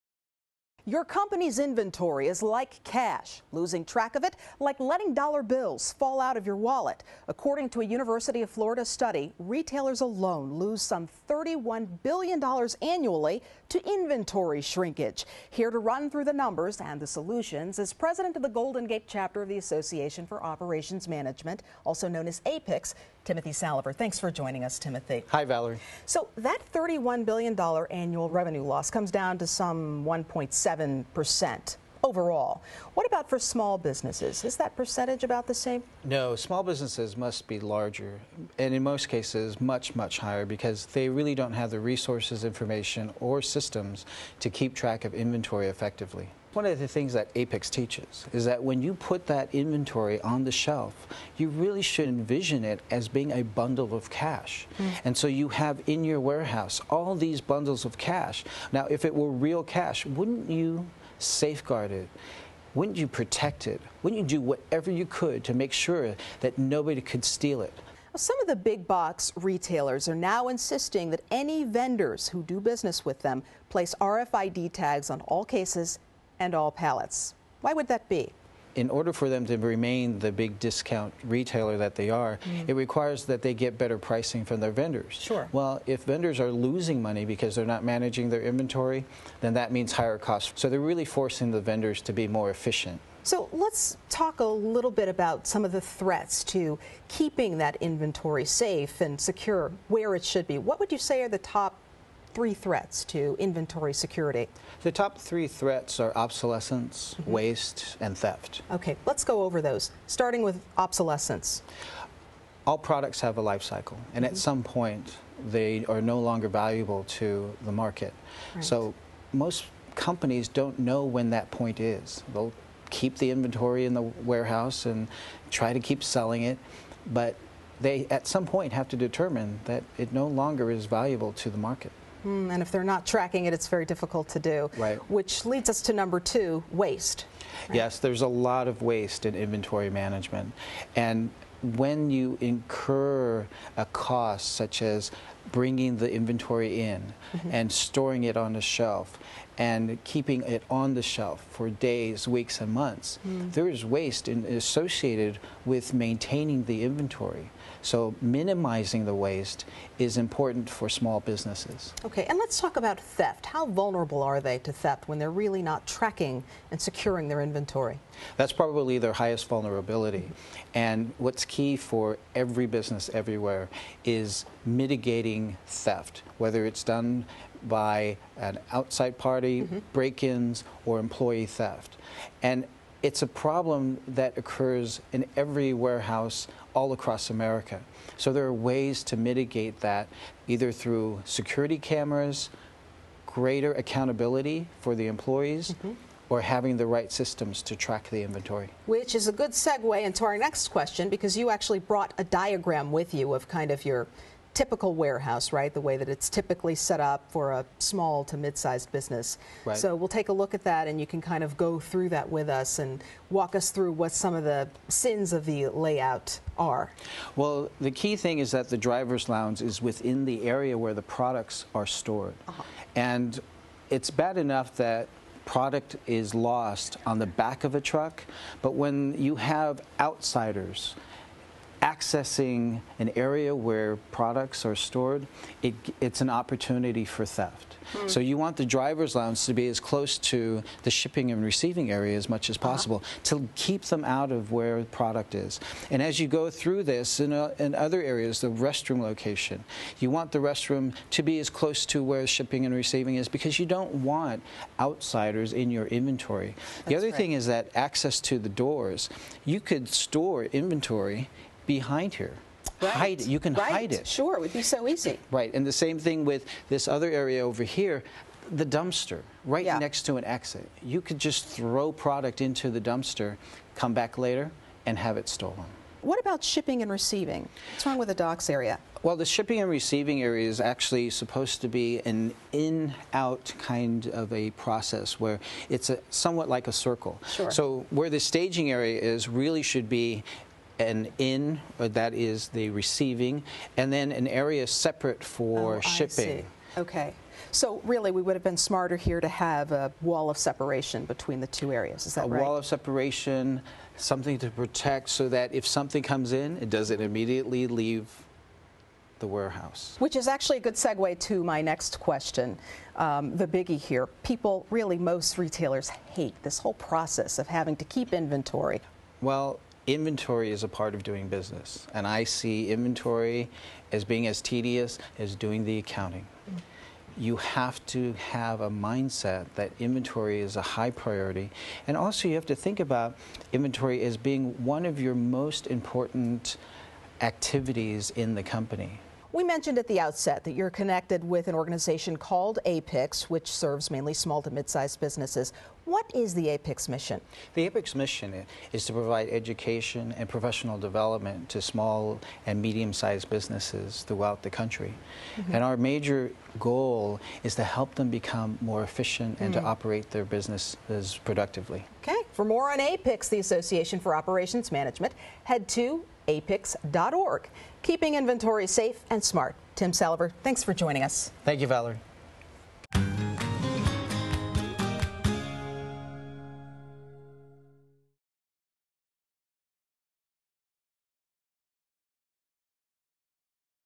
How to Make Your Inventory Safe and Smart: Innovators Forum Guest
features Timothy Salaver, President of the Golden Gate Chapter of APICS, as he outlines common mistakes that owners of small businesses make when laying out their warehouses, mistakes that put their hard-earned inventory at risk.  Discover the right way to protect your inventory in this informative, objective video.